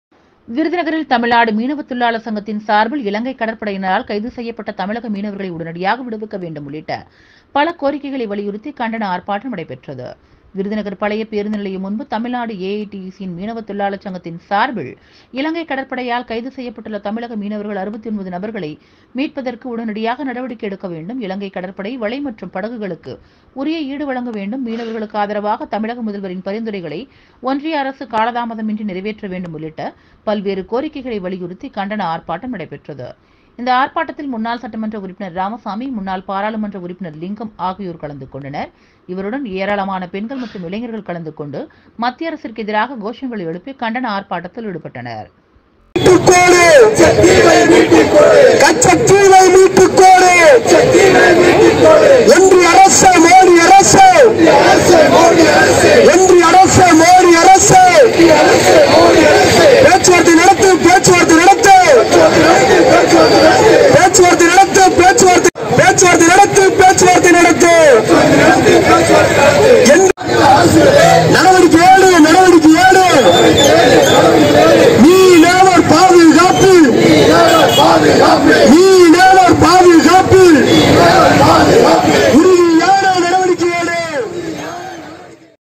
தமிழ்நாடு மீனவத் தொழிலாளர் சங்கம் சார்பில் கண்டன ஆர்ப்பாட்டம்
FIRSTNEWS_TAMIL FN_Tamil விருதுநகரில் தமிழ்நாடு மீனவத் தொழிலாளர் சங்கம் சார்பில் இலங்கை கடற்படையினரால் கைது செய்யப்பட்ட தமிழக மீனவர்களை உடனடியாக விடுவிக்க வேண்டும் உள்ளிட்ட பல கோரிக்கைகளை வலியுறுத்தி கண்டன ஆர்ப்பாட்டம் நடைபெற்றது

விருதுநகர் பழைய பேருந்து நிலையம் முன்பு தமிழ்நாடு ஏஐடியூசி மீனவத்  தொழிலாளர் சங்கம் சார்பில் இலங்கை கடற்படையால் கைது செய்யப்பட்டுள்ள தமிழக மீனவர்கள் 69 நபர்களை  மீட்பதற்கு உடனடியாக நடவடிக்கை எடுக்க வேண்டும் இலங்கை கடற்படை  வலை மற்றும்  படகுகளுக்கு உரிய  ஈடு வழங்க வேண்டும் மீனவர்களுக்கு ஆதரவாக தமிழக முதல்வரின் பரிந்துரைகளை ஒன்றிய அரசு காலதாமதமின்றி நிறைவேற்ற வேண்டும் உள்ளிட்ட பல்வேறு கோரிக்கைகளை வலியுறுத்தி கண்டன ஆர்ப்பாட்டம் நடைபெற்றது இந்த ஆர்ப்பாட்டத்தில் முன்னாள் சட்டமன்ற உறுப்பினர் ராமசாமி முன்னாள் பாராளுமன்ற உறுப்பினர் லிங்கம் ஆகியோர் கலந்து கொண்டனர் இவருடன் ஏராளமான பெண்கள் மற்றும் இளைஞர்கள் கலந்து கொண்டு மத்திய அரசுக்கு எதிராக கோஷங்கள் எழுப்பி கண்டன ஆர்ப்பாட்டத்தில் ஈடுபட்டனர்